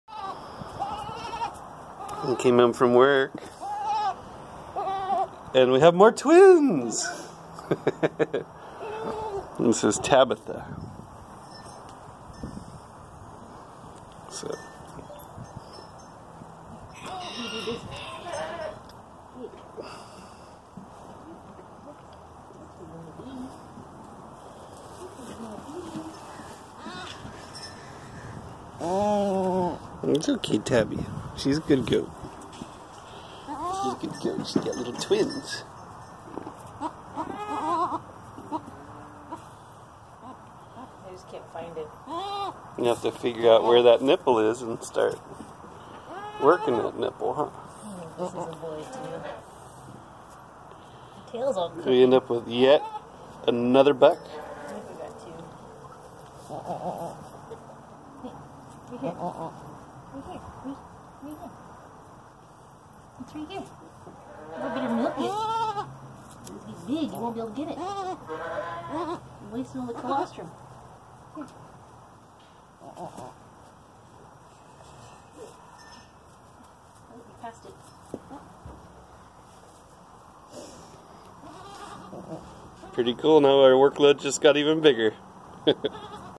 Baby goats on the Riley ranch.
Our oldest goat Tabitha had twin baby bucks! Are other doe is pregnant yep I gave her a bunch of green